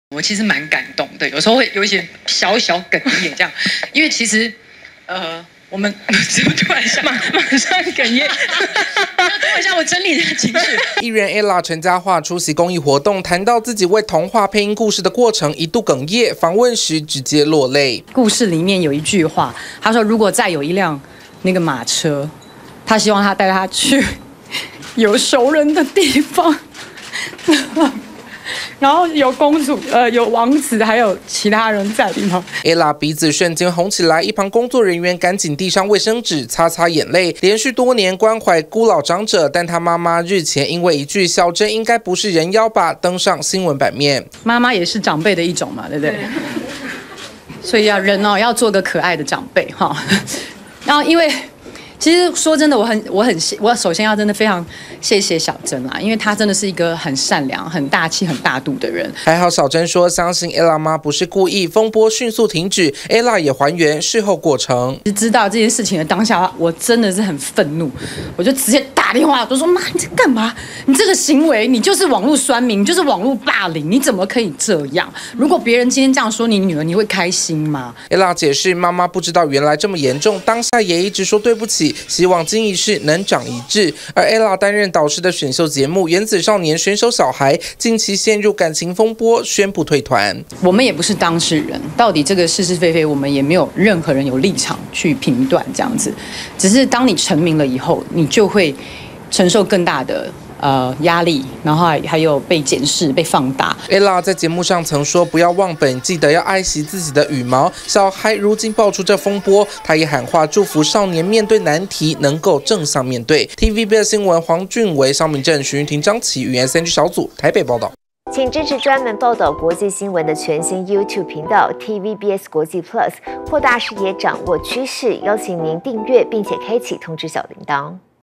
【星聞爆爆】Ella記者會突淚崩！還原「小禎人妖說」急CALL母：妳是網路酸民
藝人Ella陳嘉樺，今天（18號）出席公益活動，談到自己為公益配音的過程，情緒突然上來，數度淚崩！但隨後被問母親日前失言，留言小禎是人妖，Ella恢復冷靜還原過程，表示她當下馬上打電話，告訴母親「這個行為就是網路酸民」，也謝謝小禎沒有計較。

2013年TVBS歡樂台第二部原創概念劇